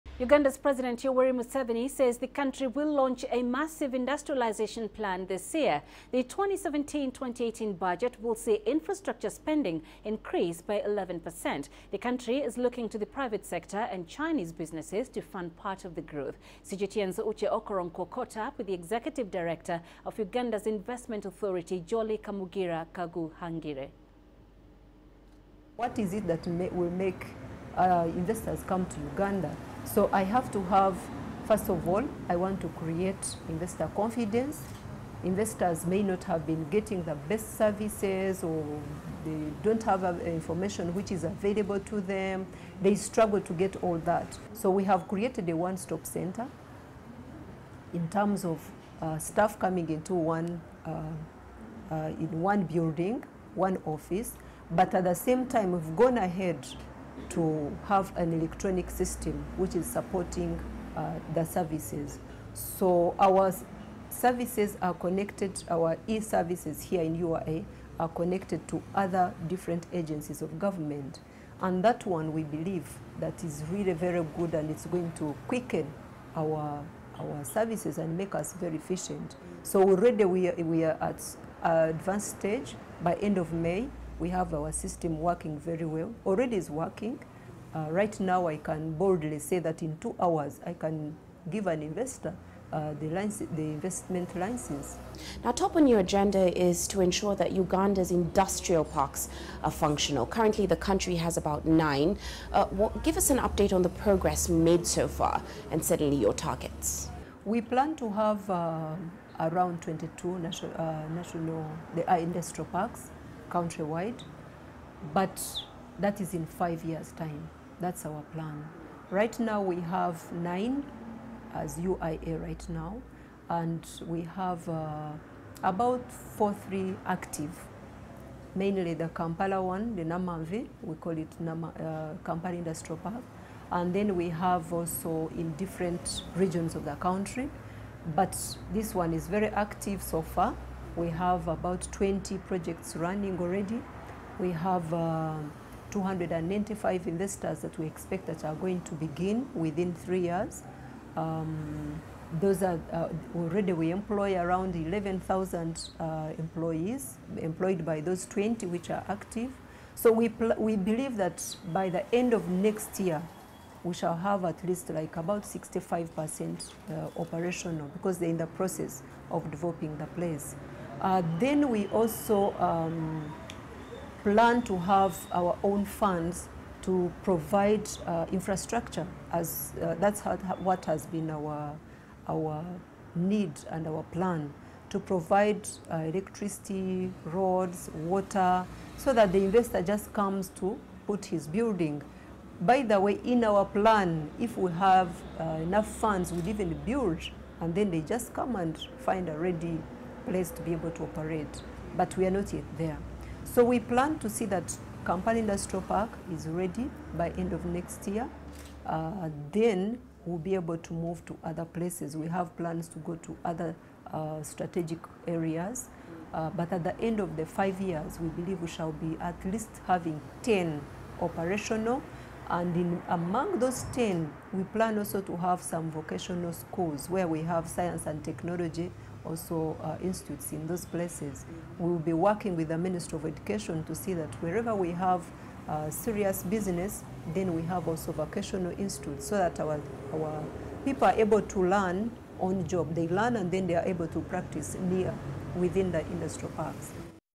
President Museveni looks to China for funding for growth plan
Uganda's President Yoweri Museveni says the country will launch a massive industrialisation plan this year. The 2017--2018 budget will see infrastructure spending increase by 11 percent. The country is looking to the private sector and Chinese businesses to fund part of the growth.  CGTN's Uche Okoronkwo caught up with the executive director of Uganda's Investment Authority, Jolly Kamugira Kaguhangire